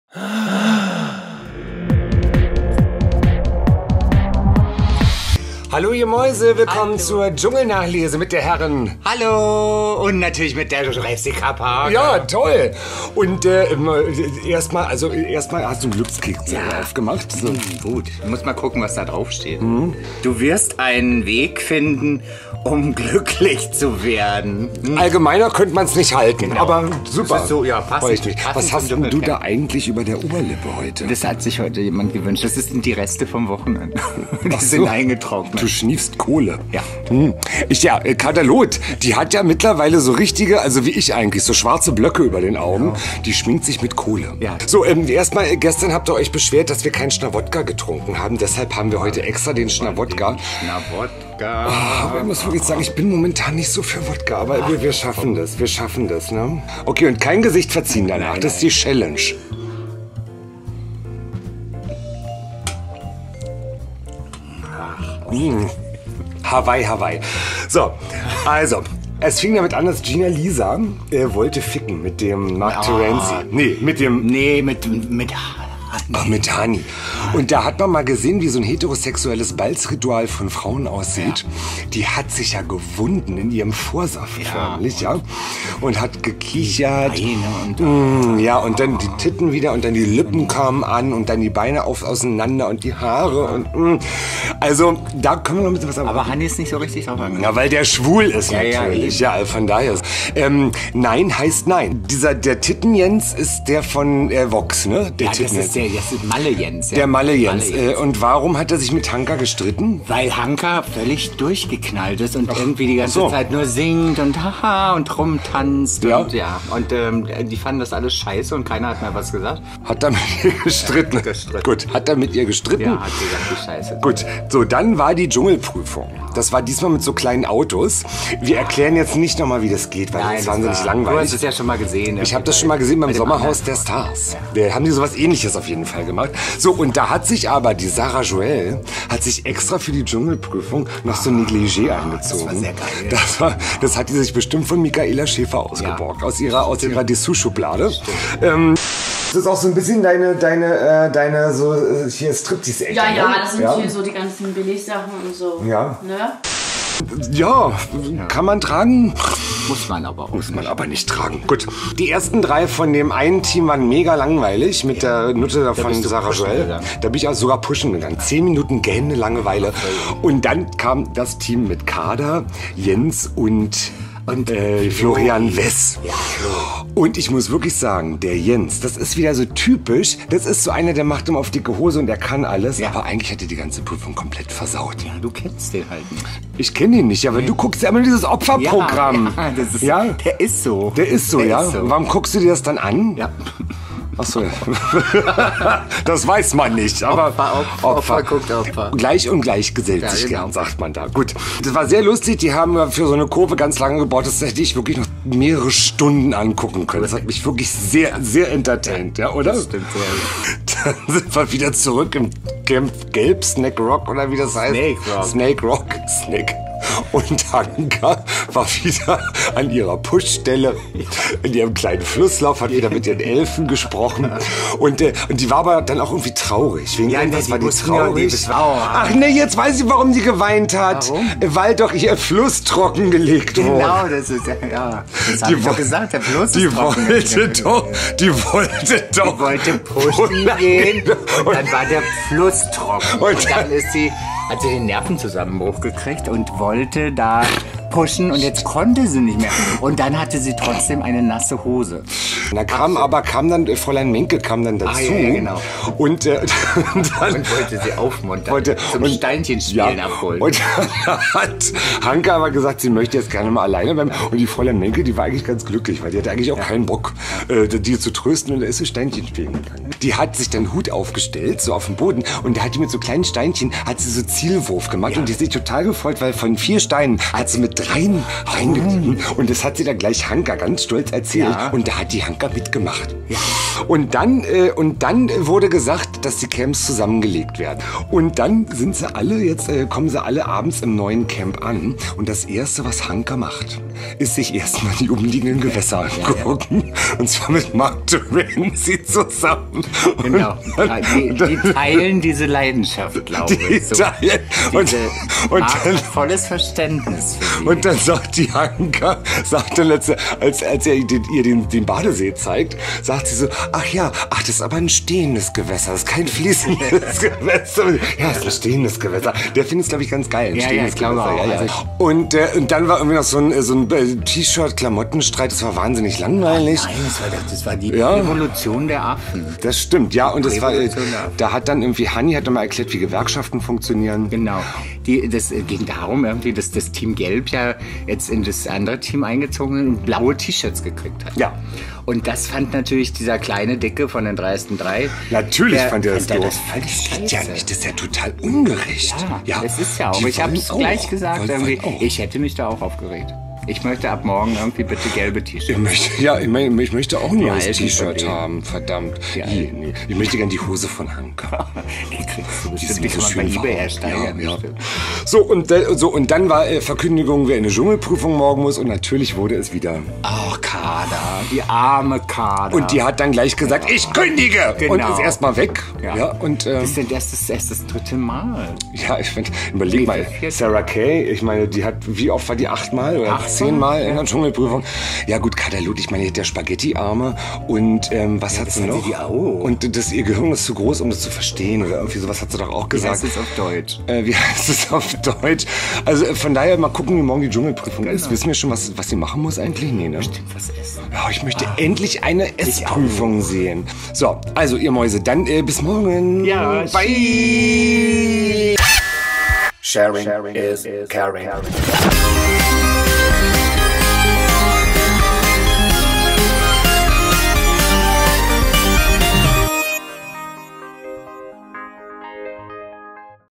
Jurassica Parka: Dschungel-Nachlese (16.01.2017)
Die aktuelle Nachlese des vergangenen Abends hier und jetzt live vom Backstage-Sofa des Rauschgolds.